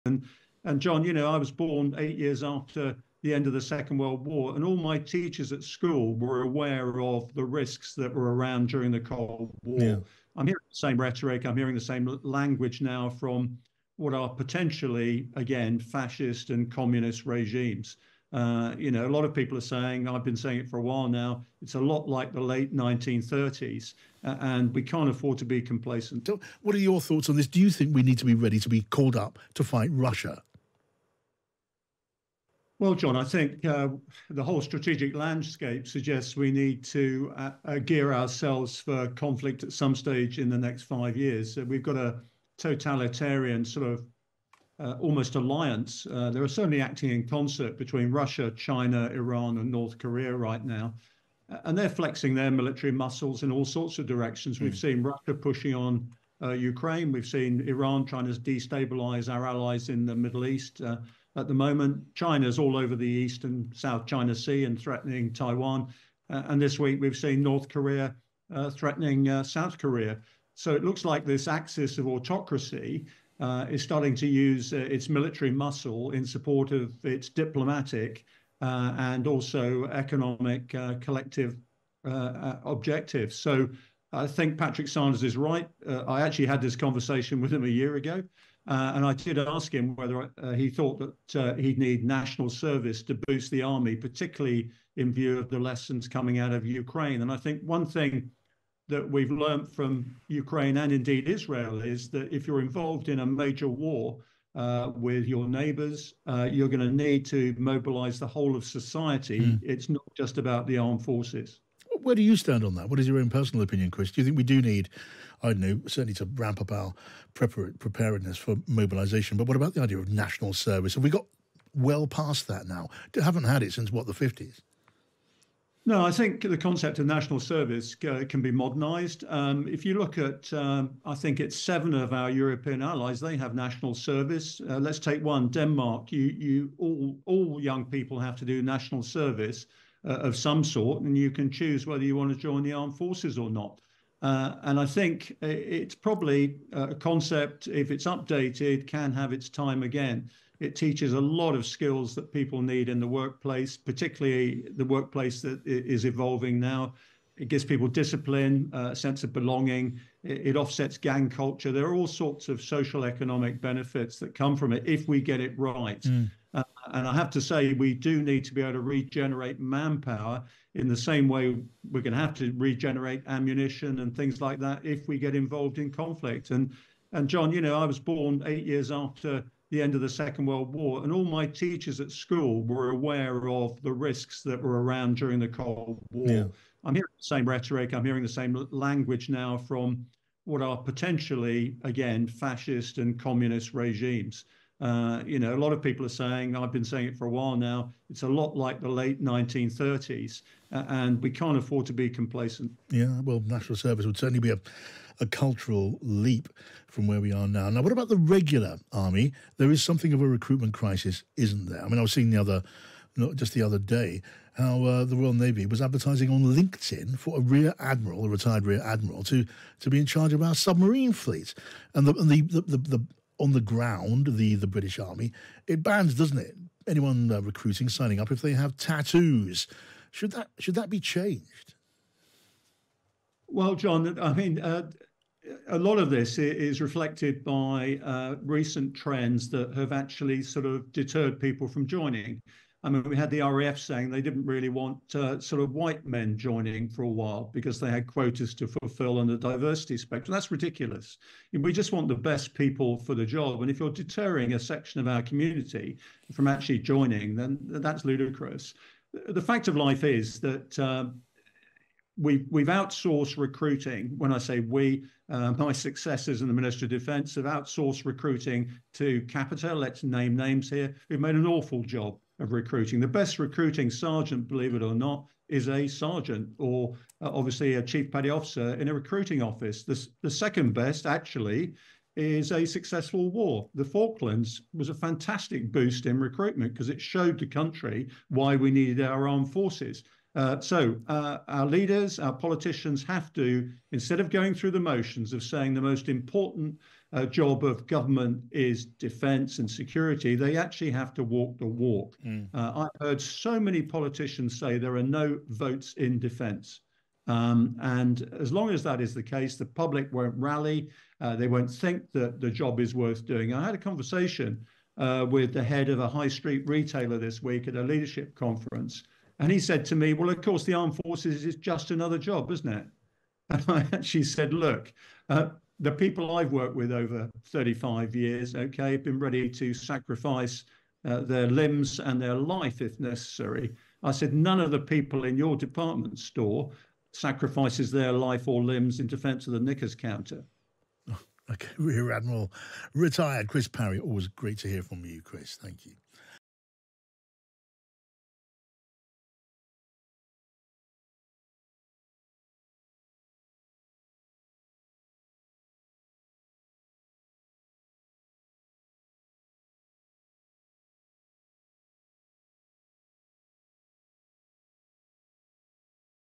Britain should 'prepare for mobilisation' against Russia | Rear Admiral Dr Chris Parry
"The concept of national service can be modernised."

A modern national service could improve the UK workforce and deter aggression from hostile nations, Rear Admrial Chris Parry tells timesradio after Gen. Sir Patrick Sanders discussed forming a 'citizen army'.